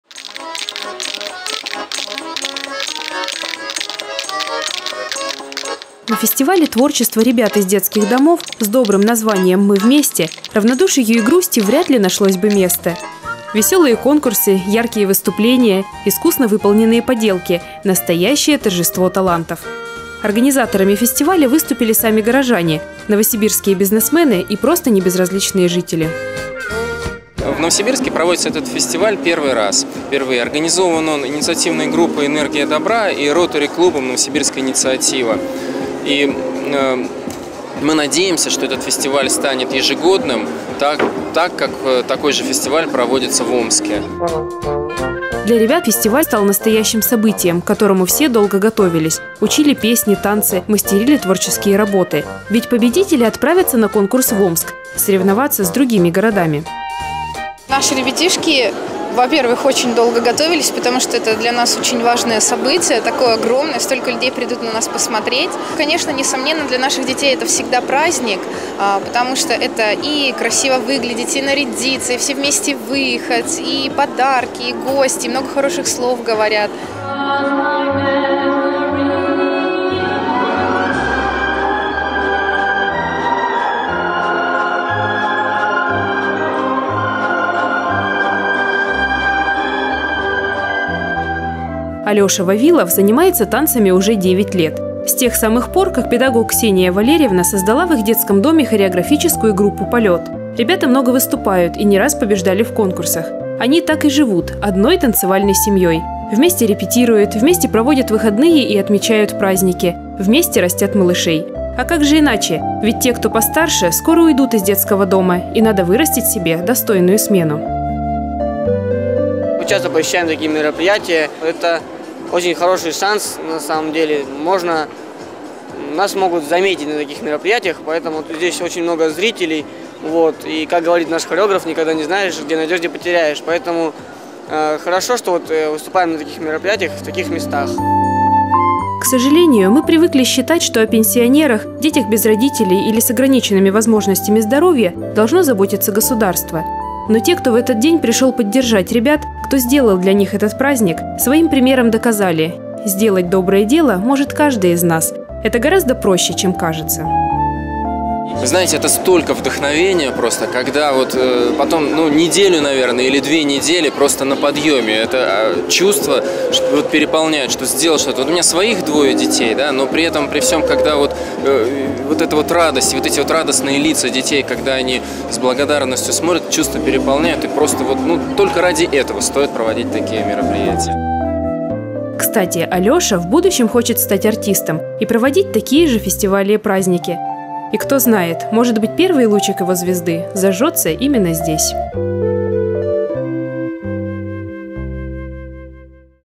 Фестиваль "Мы - Вместе", Новосибирск 28 января 2012 г
Фестиваль детского творчества среди воспитанников детских домов проведён при помощи Ротари клуба "Новосибирск-Инициатива". Материал подготовлен киностудией "Репортёр"